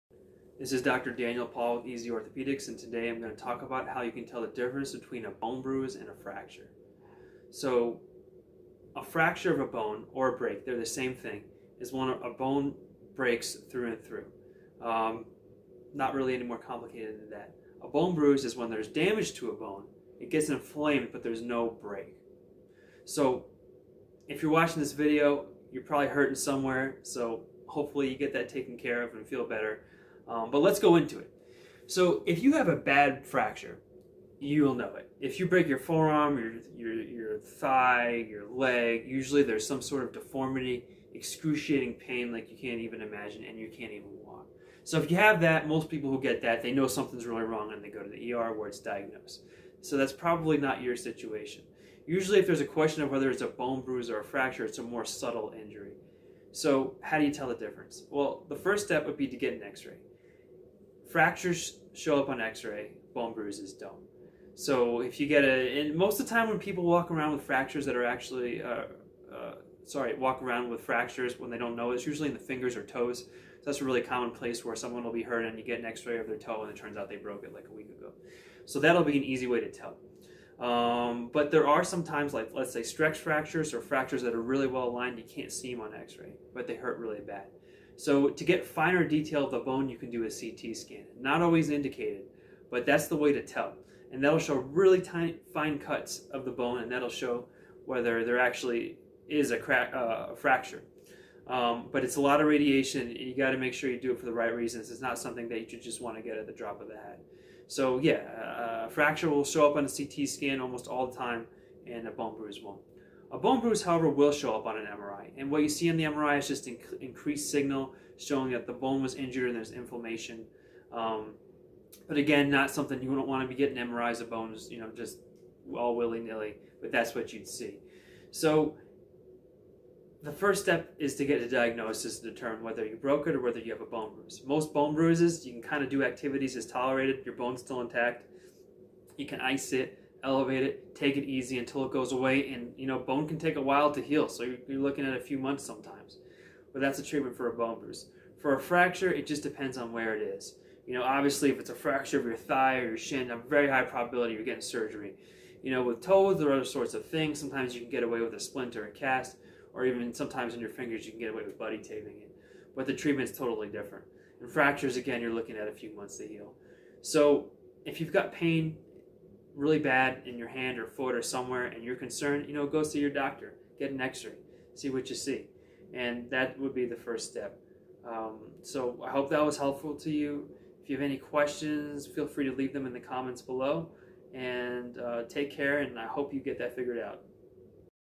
How to tell if a bone is fractured or bruised
Are you experiencing pain and not sure whether you're dealing with a bone fracture or bone bruise? Dr. Paull discusses the differences in symptoms and causes for clear bone breaks, bruises and stress fractures.